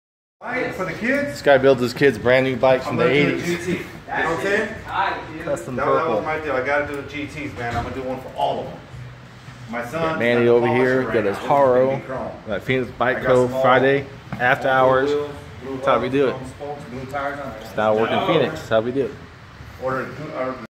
good time @ PBC bikeshop.
Ran into a few friends when I stop by last week.